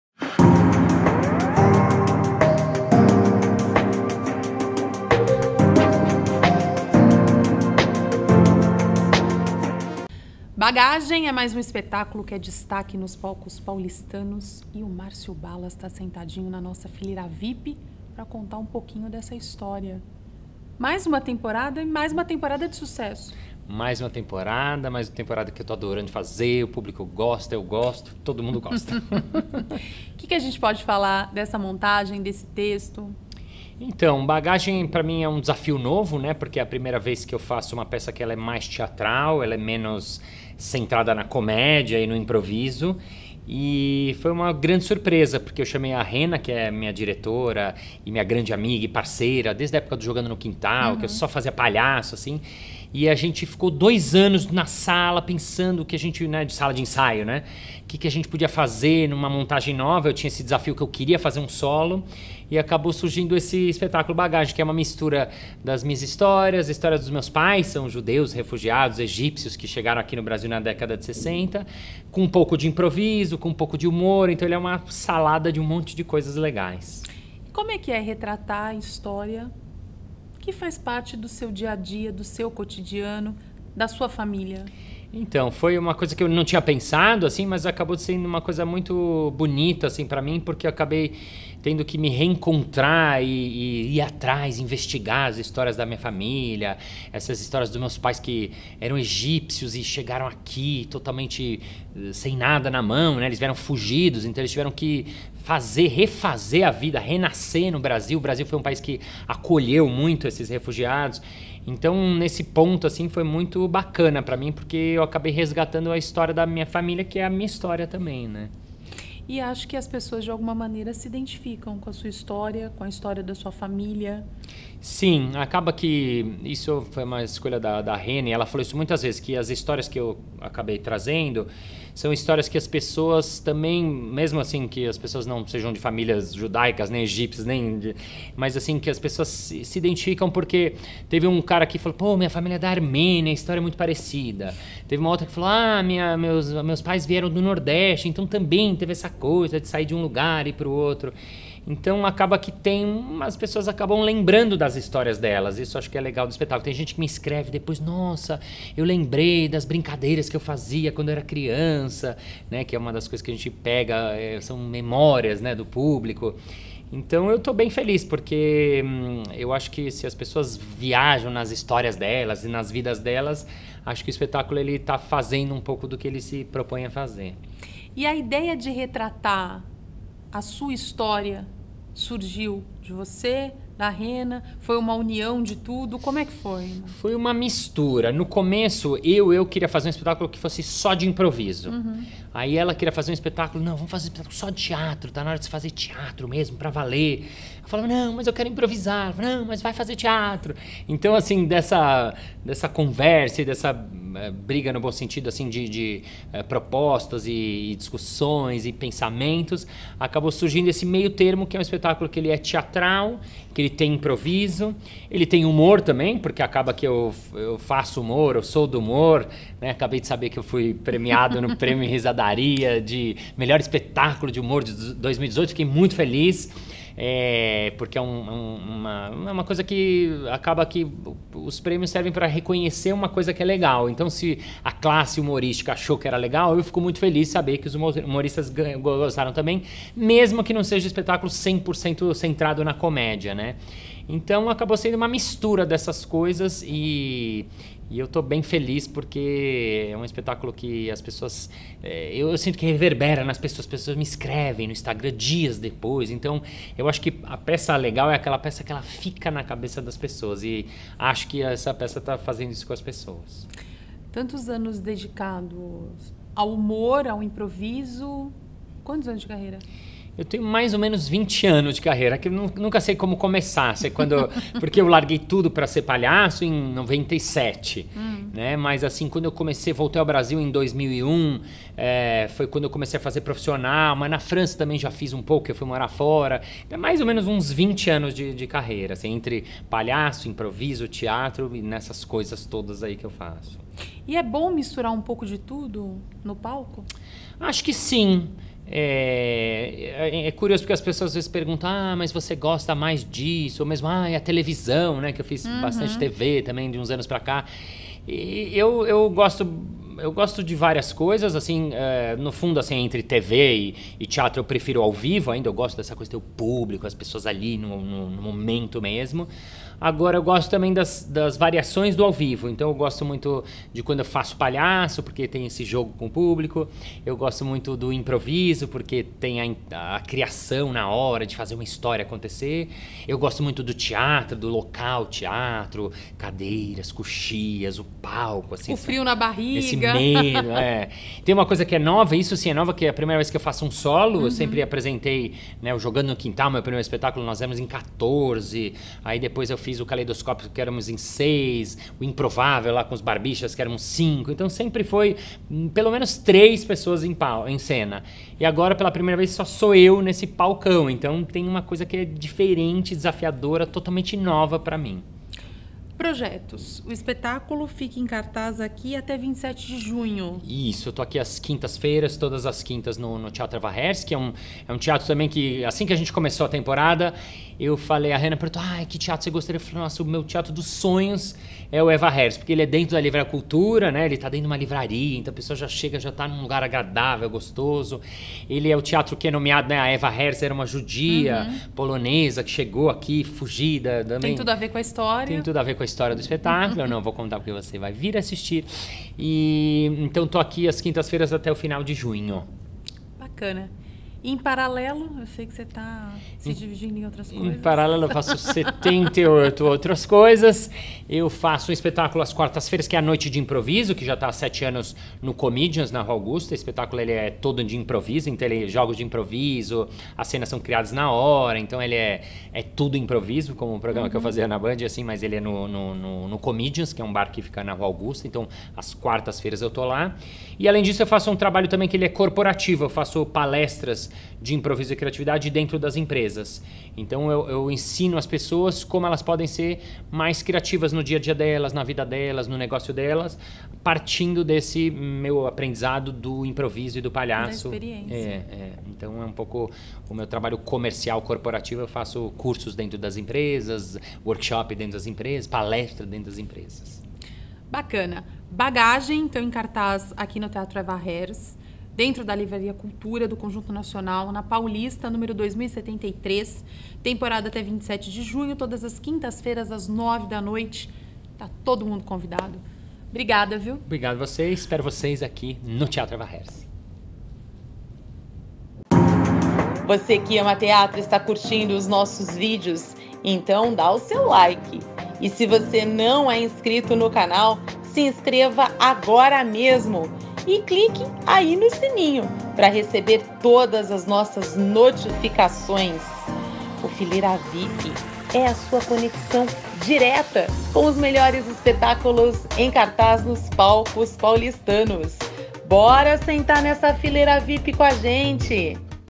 Bagagem com Marcio Ballas
Bagagem é o solo do ator Marcio Ballas, que há vinte anos vem se desenvolvendo nas linguagens do Palhaço e da Improvisação Teatral, tendo se tornado uma referência nessas áreas. Filho de imigrantes judeus egípcios, Ballas resgata, a partir de suas lembranças de infância, parte importante da história de sua família.

O resultado disto é um espetáculo divertido e singelo, que mescla poesia com boas doses de humor e improvisação, além de aproveitar uma das grandes qualidades do ator: a sua capacidade de se relacionar com a plateia, trazendo o espectador literalmente para dentro da cena.

A montagem tem texto e atuação de Marcio Ballas, dramaturgia e direção de Rhena de Faria no Teatro Eva Herz – Livraria Cultura do Conjunto Nacional que fica na Avenida Paulista, 2073, Bela Vista. A temporada segue até 27 de junho – com sessões às quintas-feiras, às 21h (no feriado do dia 20/6 a sessão acontece às 19h) - Ingressos: R$60.

Apresentação: Cris Santos

Voz off: Cris Santos

Direção, filmagem e edição: Letiares Vieira

Produção: Cris Santos